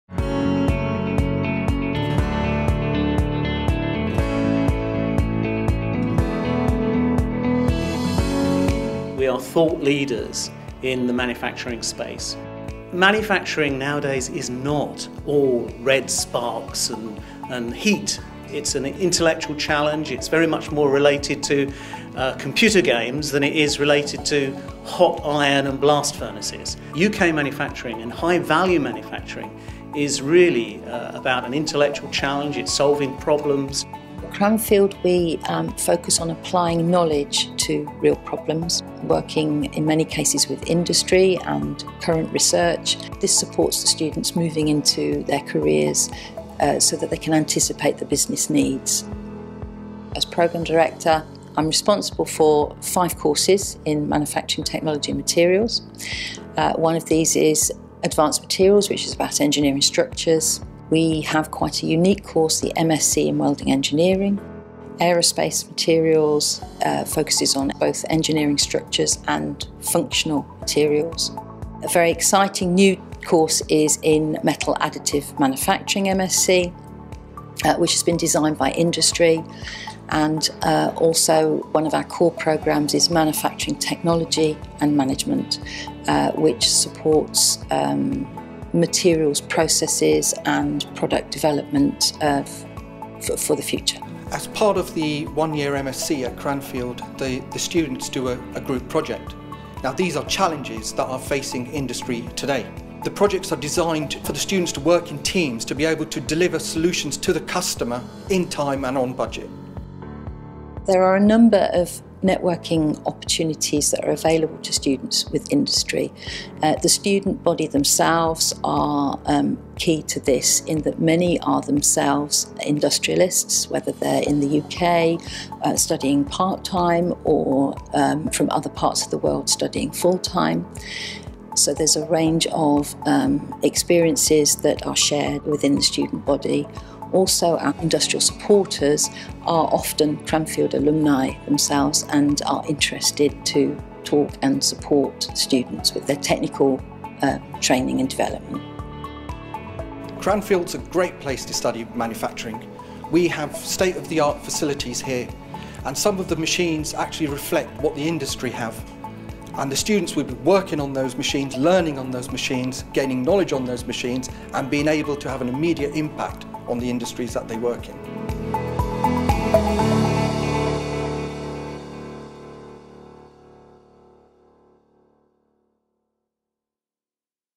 Why study manufacturing technology and materials?
This video showcases manufacturing materials at Cranfield University, highlighting Manufacturing MSc courses and our state-of-the-art facilities. Our Programme Directors describe why a Cranfield course is unique and the great benefit to career progression for prospective students.